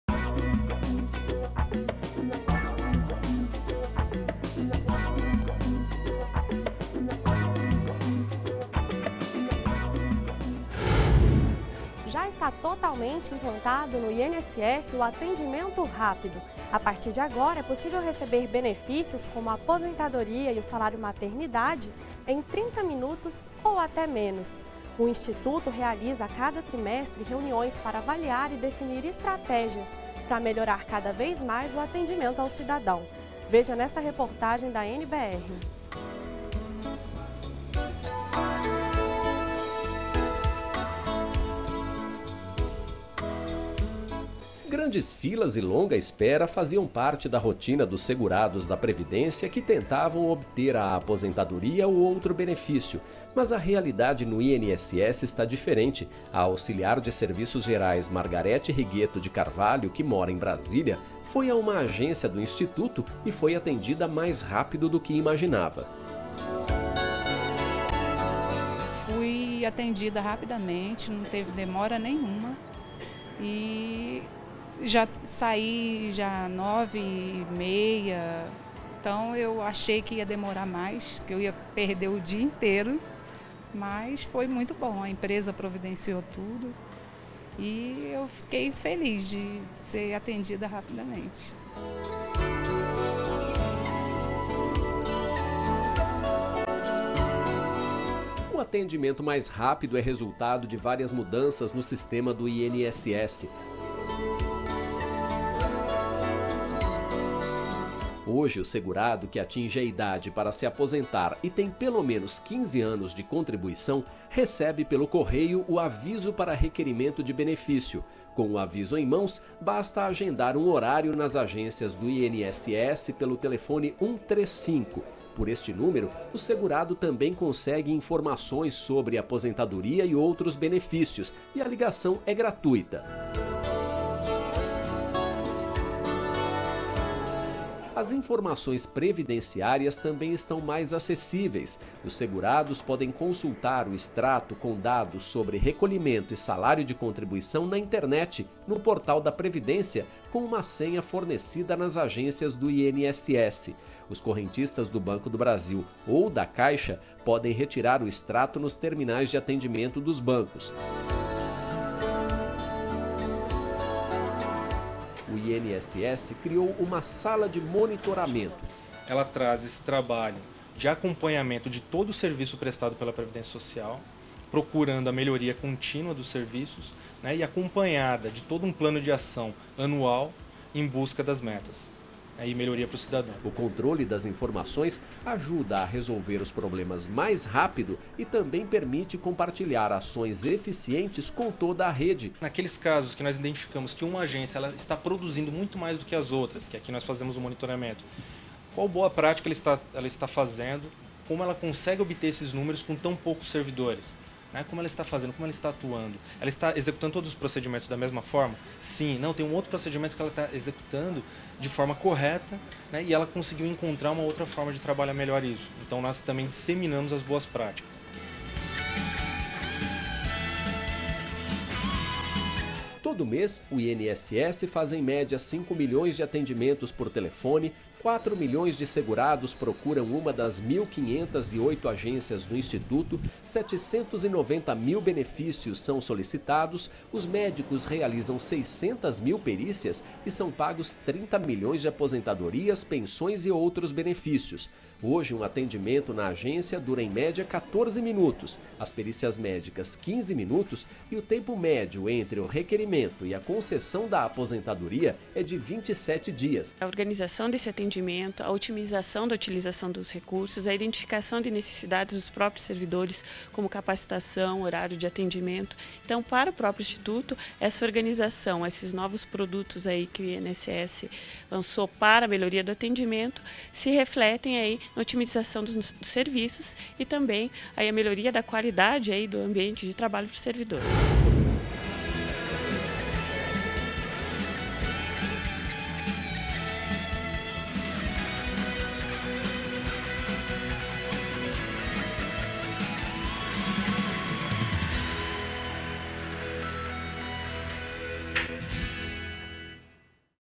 INSS otimiza prestação de serviços e torna atendimento mais rápido
DA REDAÇÃO - 30.09.13: Já está totalmente implantado o atendimento rápido no Instituto Nacional do Seguro Social (INSS). Com as mudanças, é possível receber benefícios como aposentadoria e salário maternidade em até 30 minutos, ou menos. O acompanhamento dos atendimentos é feito em uma sala de monitoramento que busca, continuamente, a melhoria dos serviços. As informações previdenciárias também estão mais acessíveis. Os segurados podem consultar o extrato com dados sobre recolhimento e salário de contribuição pela internet, pelo site da Previdência com uma senha fornecida nas agências do INSS.